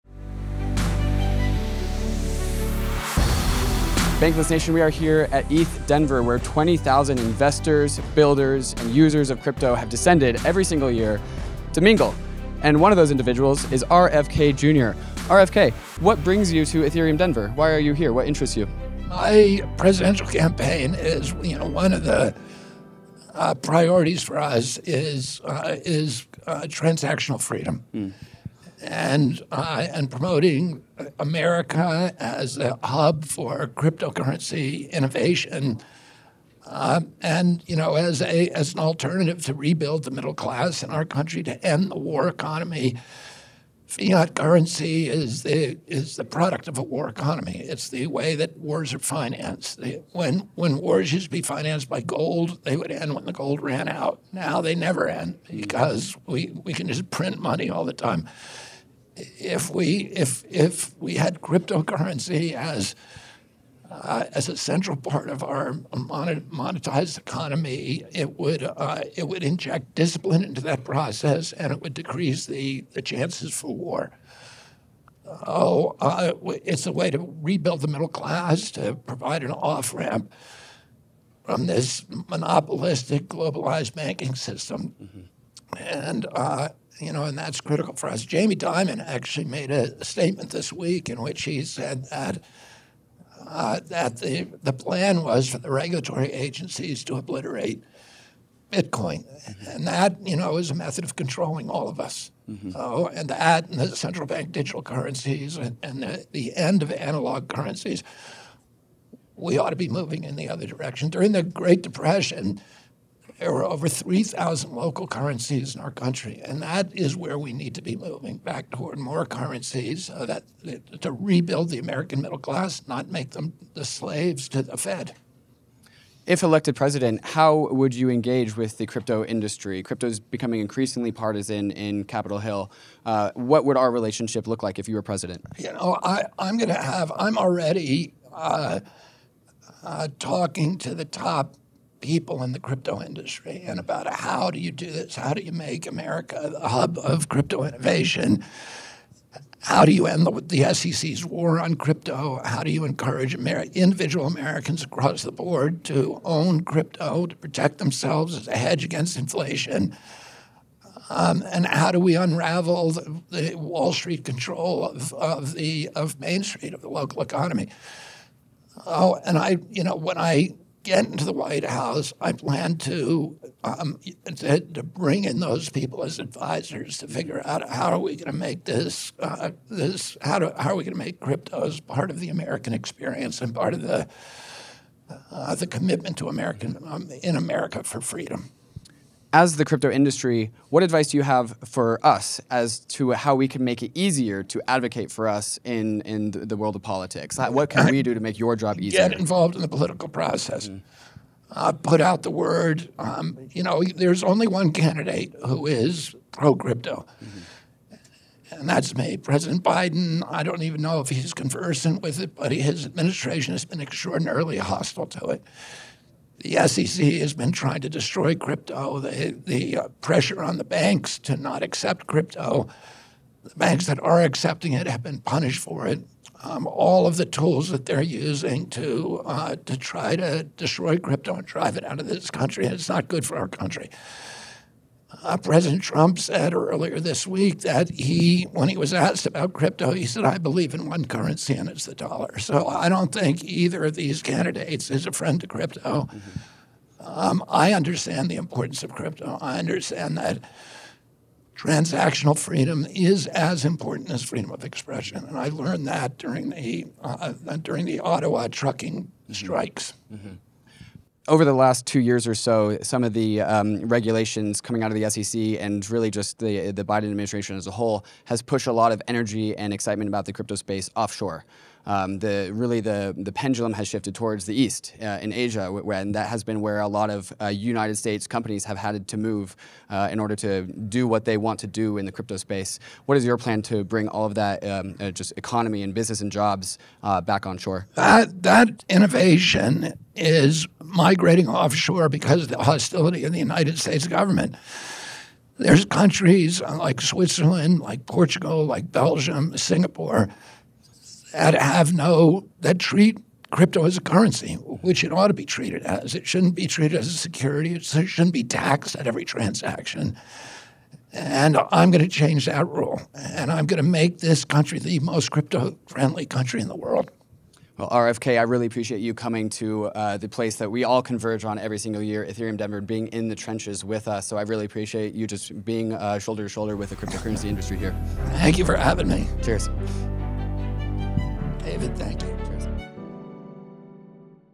Robert F. Kennedy Jr.'s Official Crypto Policy
An in person interview with presidential candidate RFK Jr. who shares his takes and policies on the state of crypto in the United States.